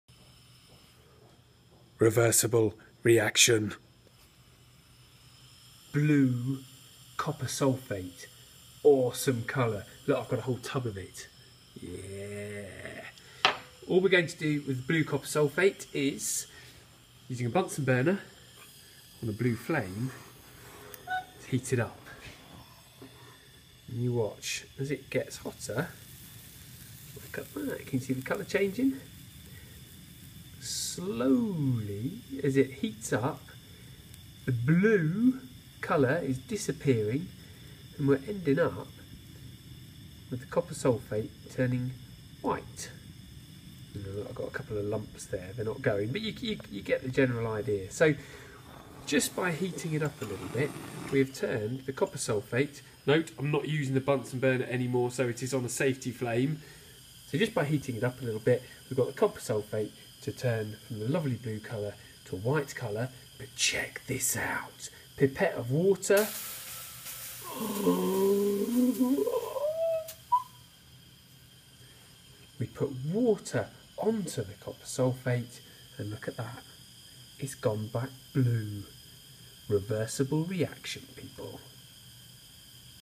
Example of a Reversible Reaction
JRO demos a reversible reaction. What questions could you ask to find out more about this and, indeed other, reversible, reactions?